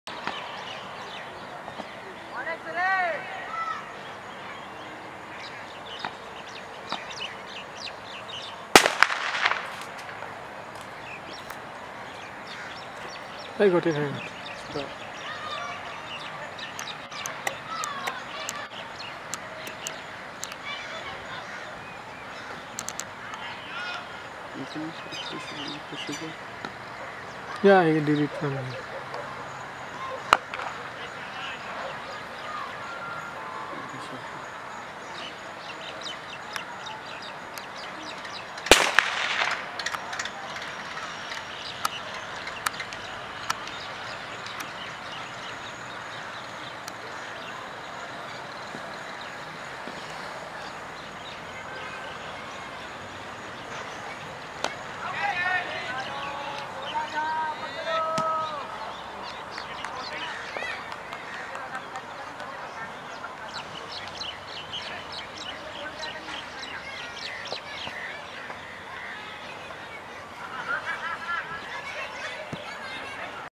Charan is dismissed! Simply One vs 7 Miles
A dismissal clip from the LiveSport Festival League Match 28: 7 Miles vs Simply One